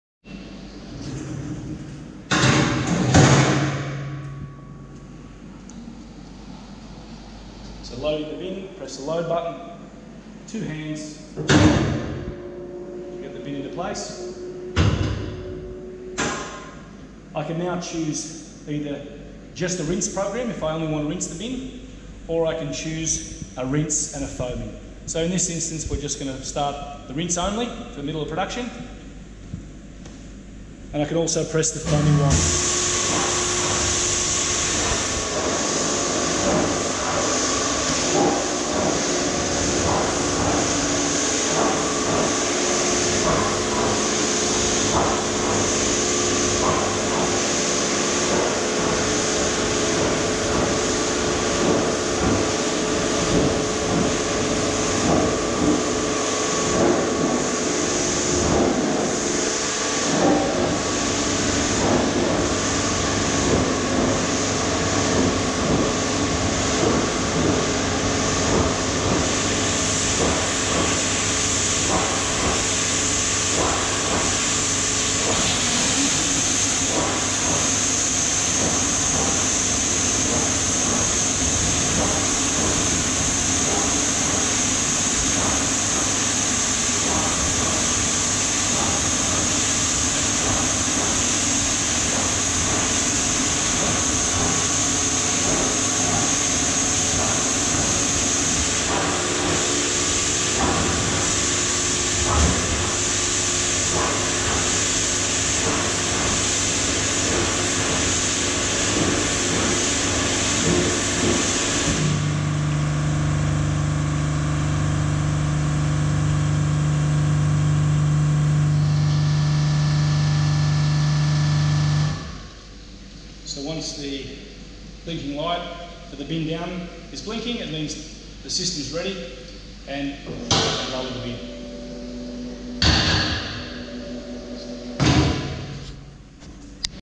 Walter - KW200 Bin Washer
With the WALTER KW200 Euro trolley cleaning system you can effortlessly optimise your working processes. A reduction in the number of circulating trolleys reduces the space required for clean and dirty trolleys. This reduced number of trolleys maximises floor space and has the labour-saving advantages of less trolleys to clean. With this reduced footprint the system can be located more centrally in your work-space.

With a weights of 43kg, unless you have an automated cleaning solution, it takes a lot of back bending, labour and time to wash 200L Euro Bins thoroughly inside and out.

Walter from Germany, manufacture high quality cleaning systems that are economical, reliable and environmentally friendly. It takes 5 minutes on a cleaning cycle for a 200L Euro Bin wash.  With push button controls - automate your bin cleaning.

The Walter's pivot arm raises each 43kg stainless steel bin in front of the pressure cleaner head for a comprehensive clean. The lift mechanism prevents dents and damage to bins on raising and lowering.